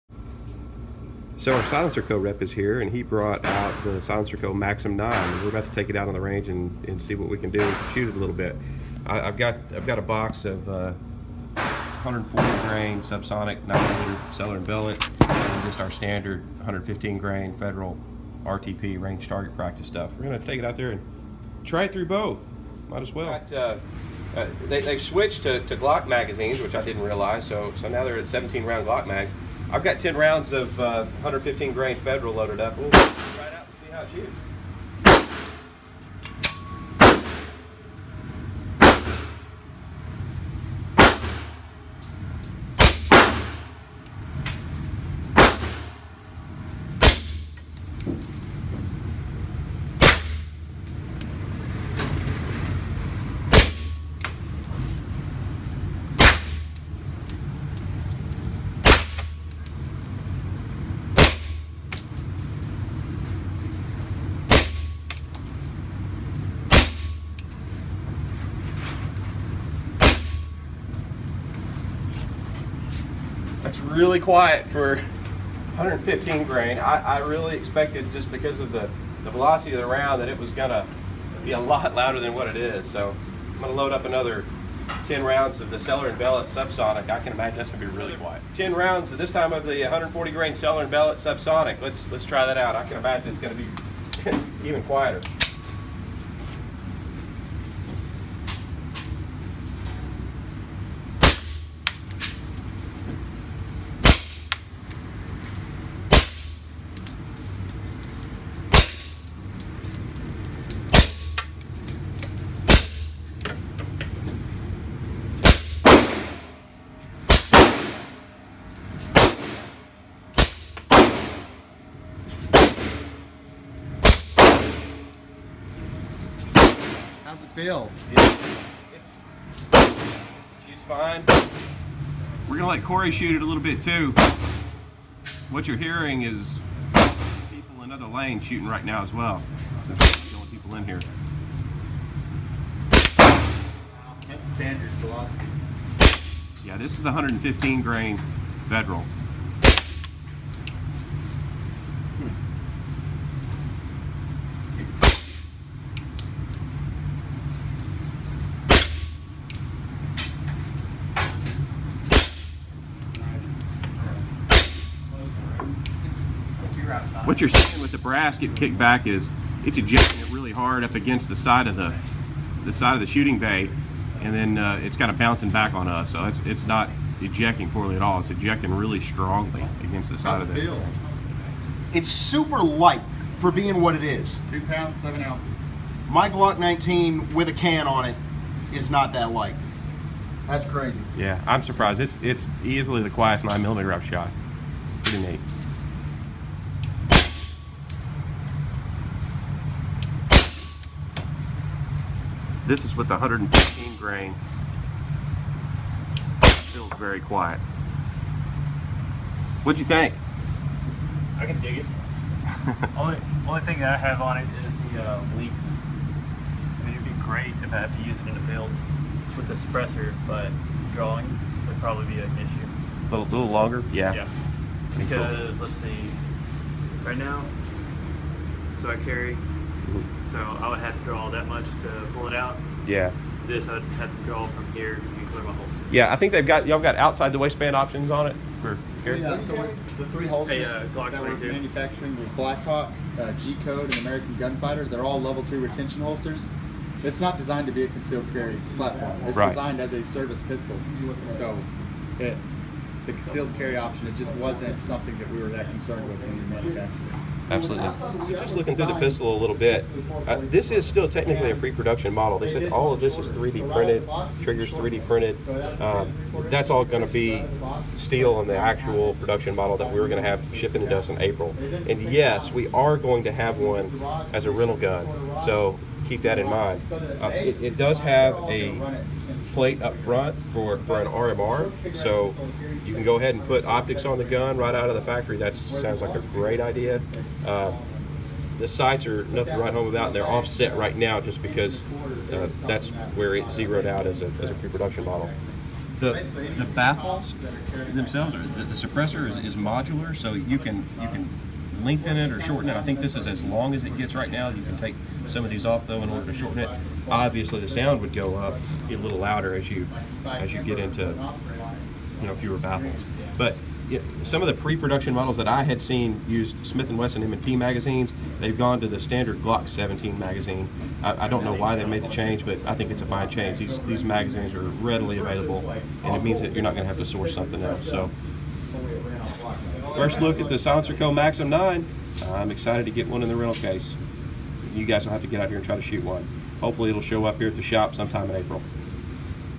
First Shots with the SilencerCo Maxim 9
Arkansas Armory's SilencerCo Rep came by the shop on 2-7-2017 with a Maxim 9.  We took it out on the range to try it out.